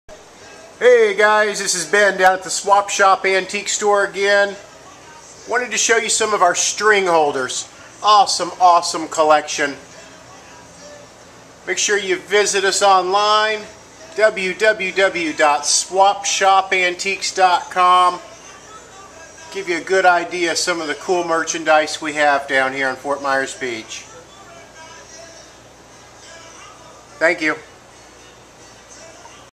String Holder String Holder at The Swap Shop Antique Store
String Holders at The Swap Shop Antique Store on Fort Myers Beach, FL. 33931. Store hours 10-5 mon-sat.